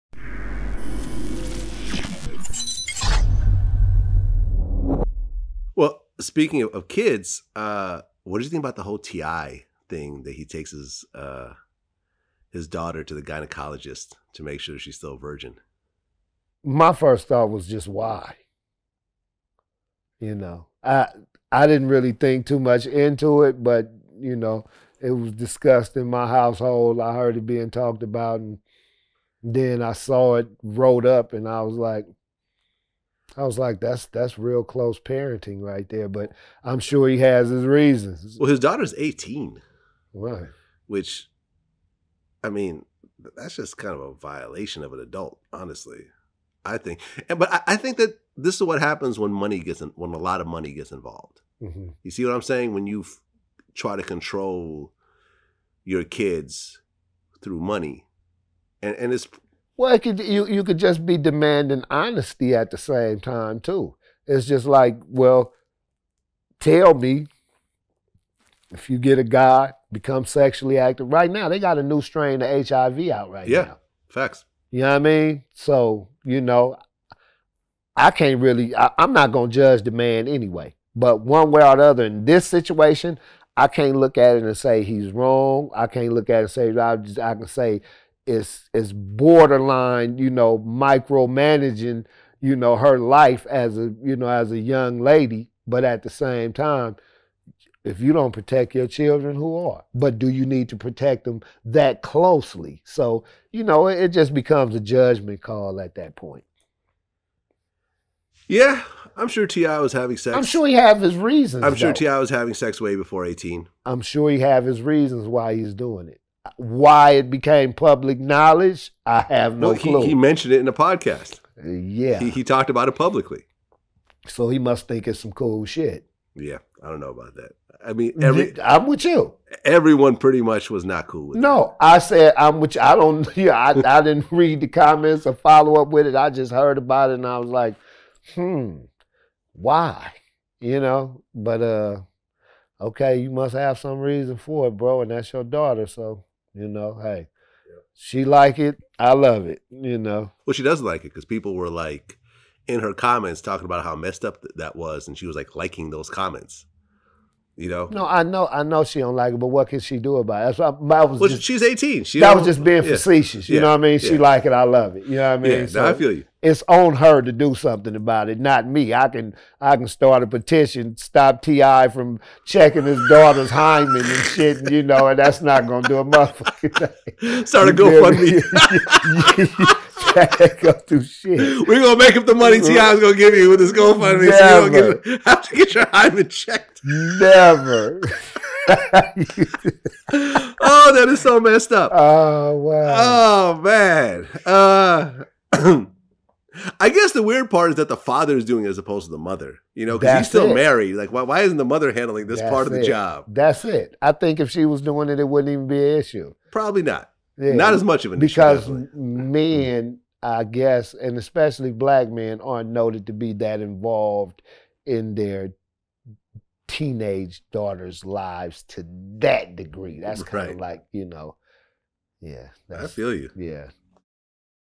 Tray Deee on T.I.'s Comments About Checking Daughter's Virginity: "Why???" (Part 13)
Tray Deee shared his thoughts on T.I.'s recent comments about taking his daughter to the gynecologist to check to see if she was still a virgin. Tray Deee said his first thought was, "Why?," but he added that Tip could've been protecting his daughter the best way he knew how. Tray Deee then pointed out that there is a new strain of HIV that was discovered in 2019, making it more of a reason for Tip to feel the need to protect his daughter. Tray Deee ended the discussion by saying that it's on T.I.'s daughter, who is now 18, to do something about the situation. To hear more, including Tray Deee's thoughts on men not being in their teenage daughters' lives, hit the above clip.